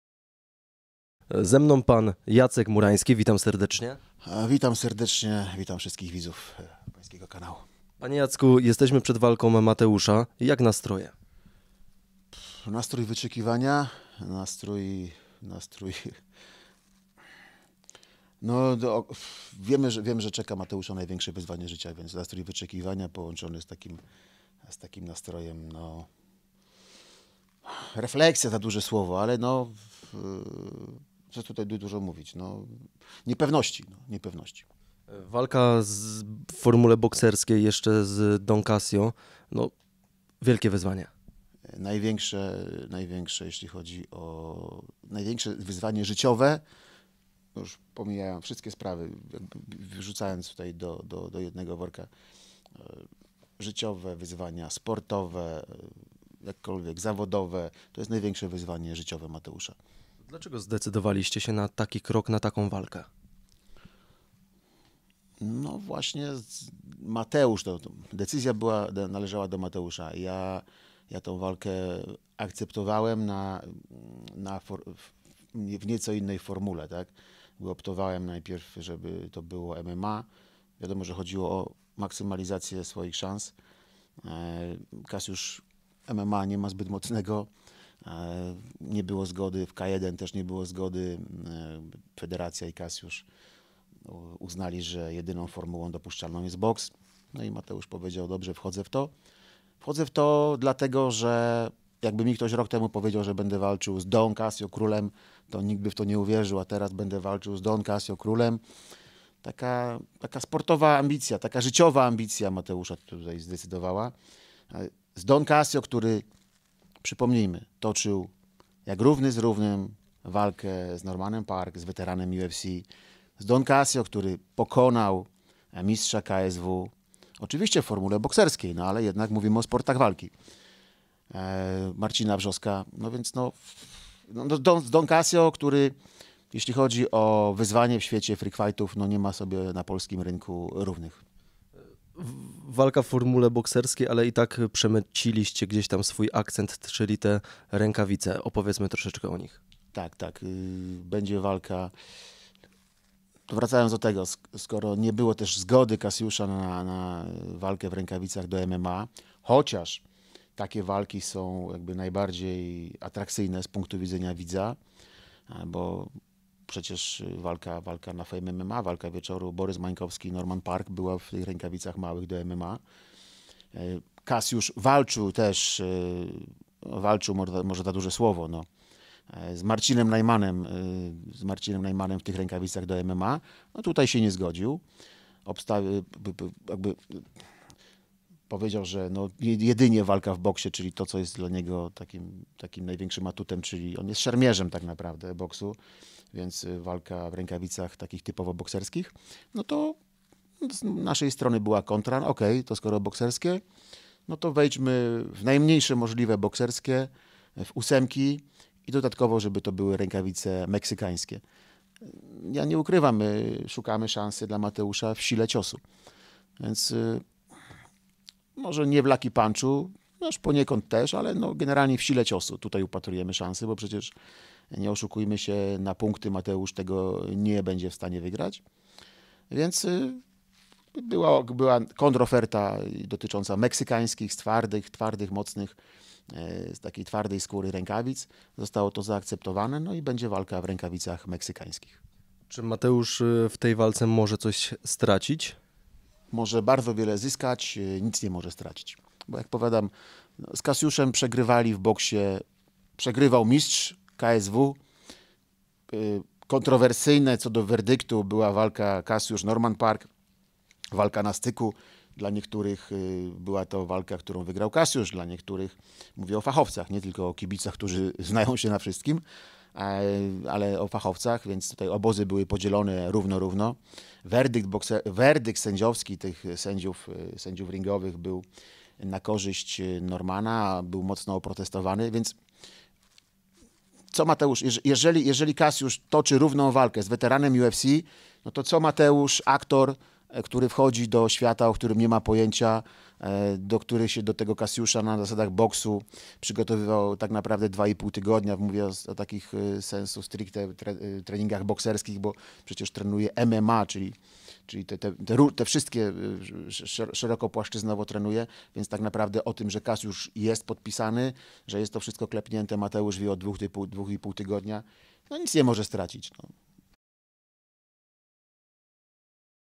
JACEK MURAŃSKI O WALCE SYNA Z DON KASJO
Jacek Murański o walce swojego syna Mateusza z Donem Kasjo na Prime MMA.